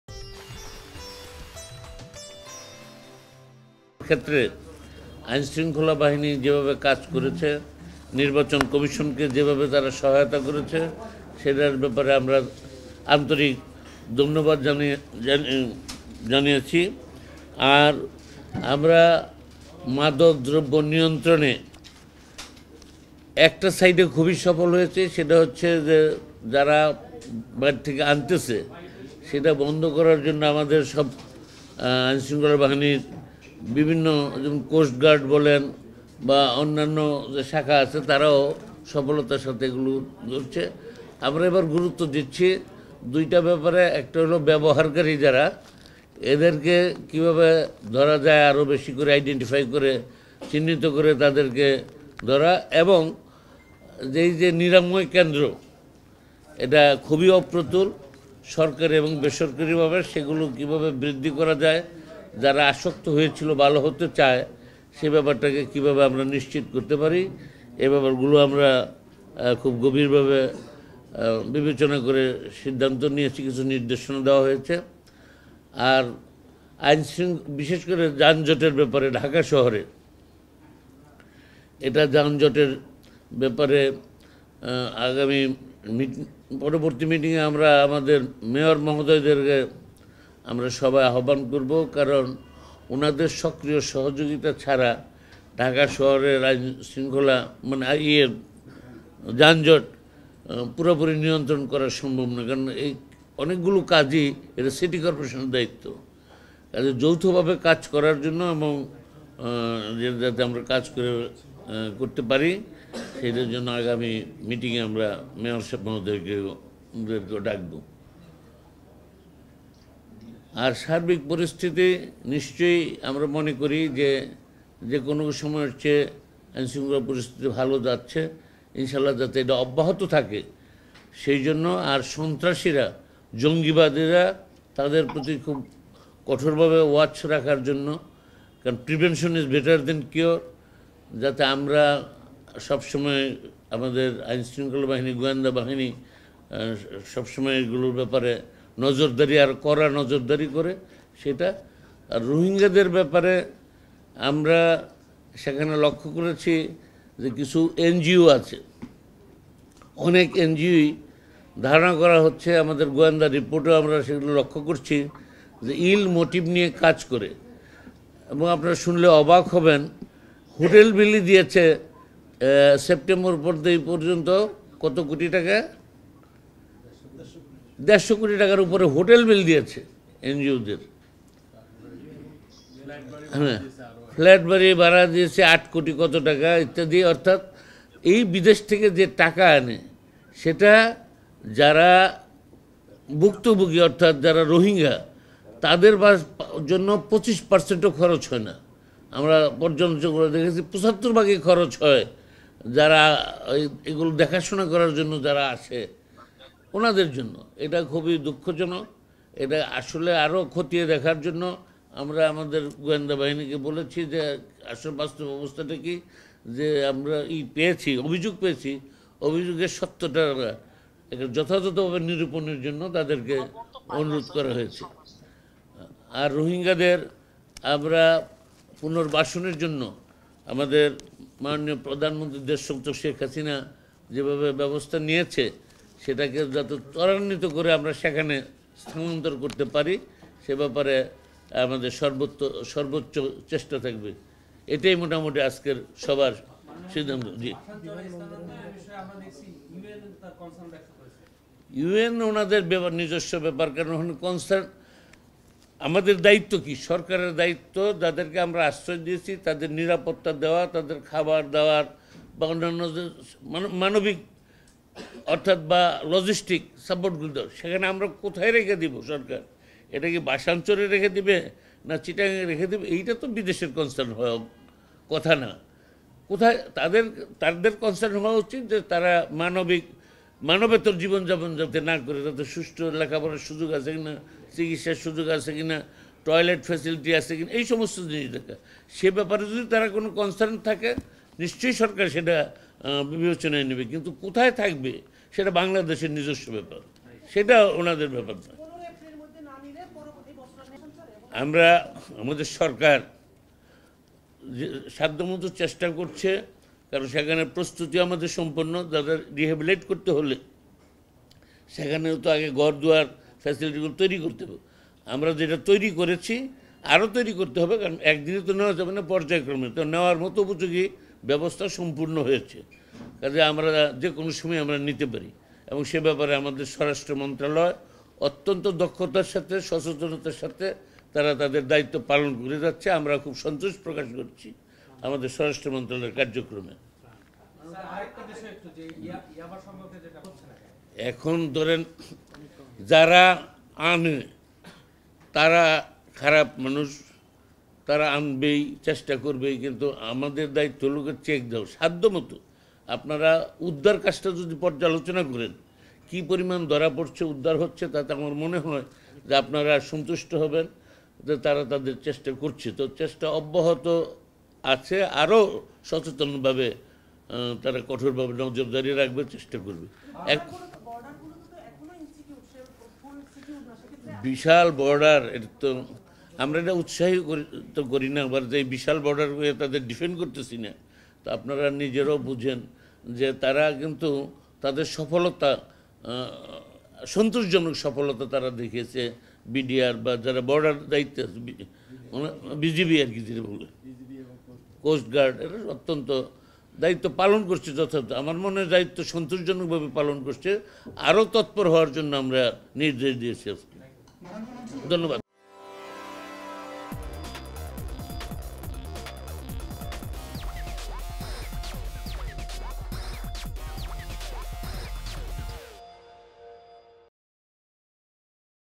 কক্সবাজারে এনজিও কর্মীদের হোটেল বিল দেরশো কোটি টাকা! | রোহিঙ্গাদের না দিয়ে নিজেদের পিছনেই খরচ করছেন
কক্সবাজারে এনজিও কর্মীদের হোটেল বিল দেরশো কোটি টাকা! | রোহিঙ্গাদের না দিয়ে নিজেদের পিছনেই খরচ করছেন। রোহিঙ্গাদের জন্য এনজিওদের আনা টাকা ২৫ ভাগও খরচ হয়না রোহিঙ্গাদের কল্যানে। Somoy TV